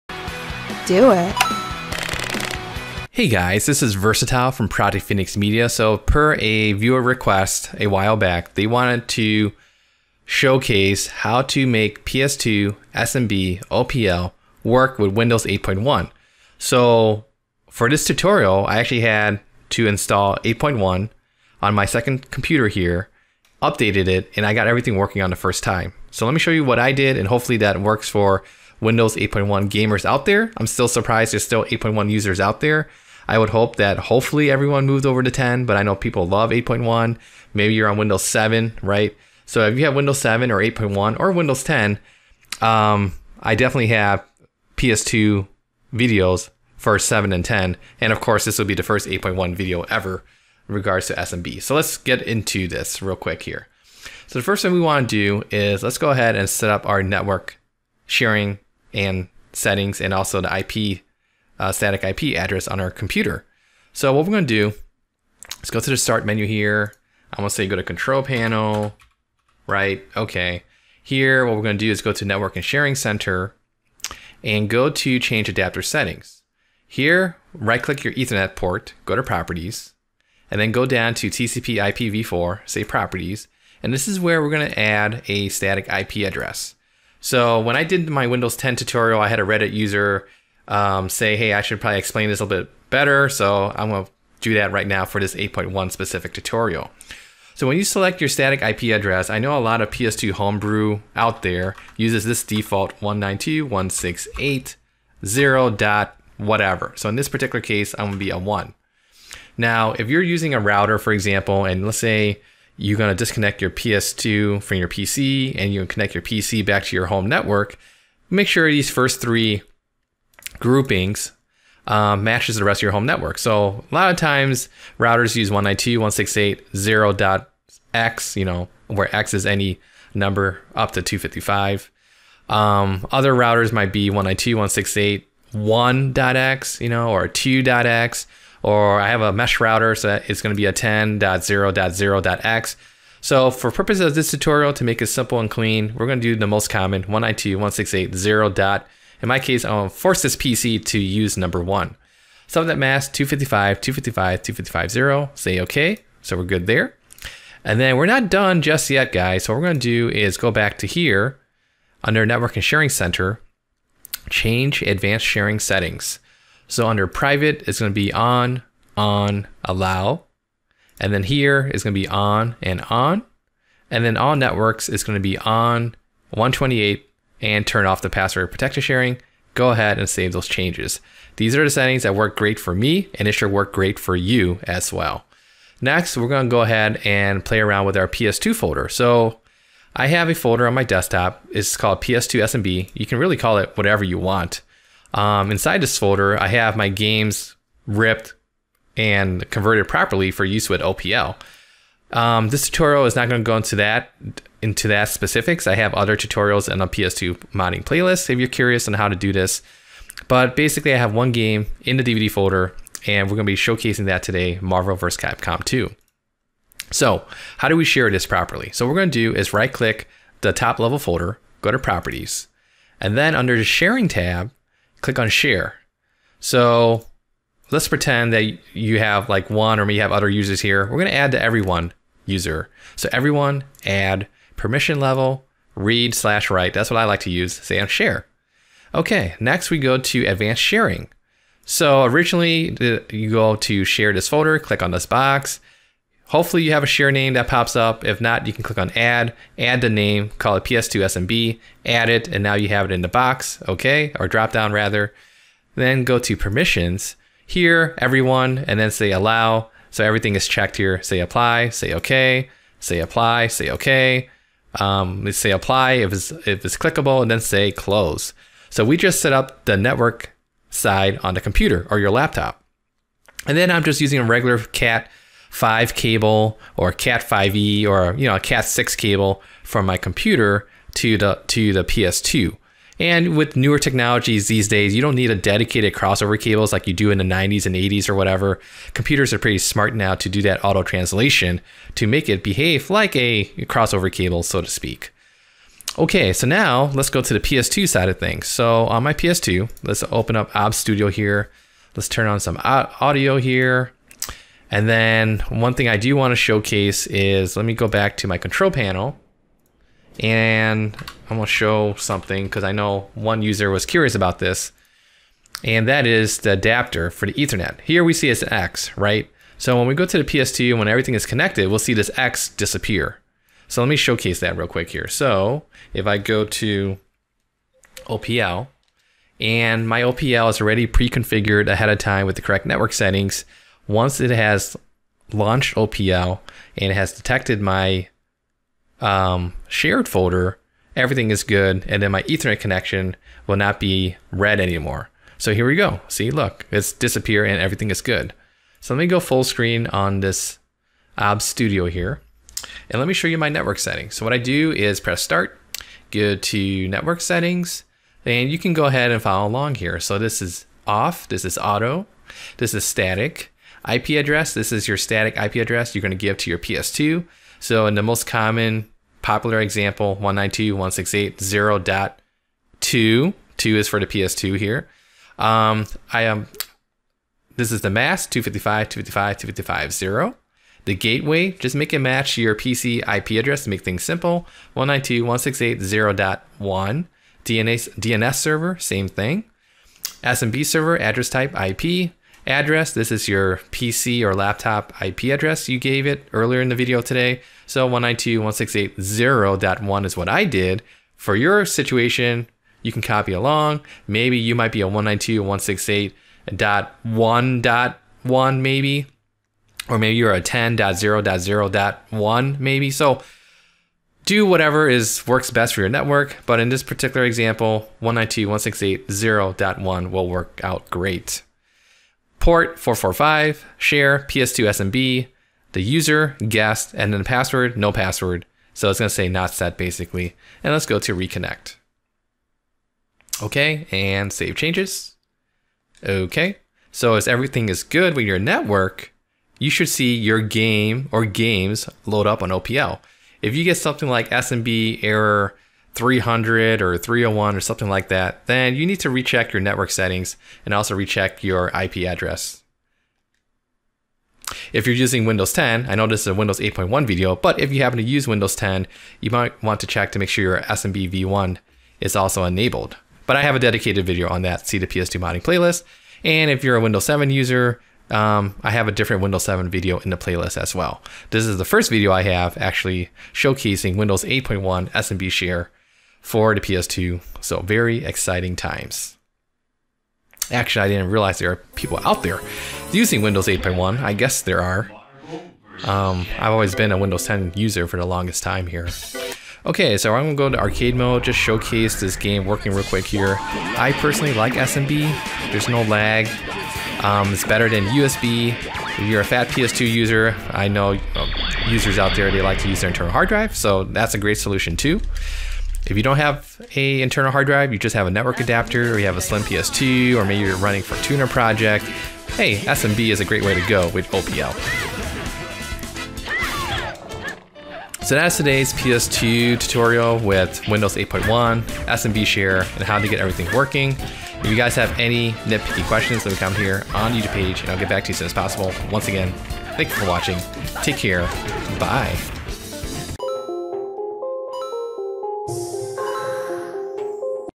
Share OPL PS2 Games Using SMB Ethernet and Windows 8.1 Tutorial (2020)
A viewer had asked awhile back to make a Windows 8.1 SMB sharing tutorial for the PS2 and OPL. I actually went back into my archives, found my Windows 8.1 disc, installed it fresh with all latest updates, connected an Ethernet cable between PC and PS2, did what I did in this video tutorial, and I got it working first time.

I don't understand why people continue to have networking issues in general, so my hope is this video gives some insight and fixes your issues too if you are on Windows 8.1. Happy Gaming!

*Links For the Video*
[None]

Needs a VPN? We recommend Private Internet Access (PIA)

MoNeYINPHX PC Specs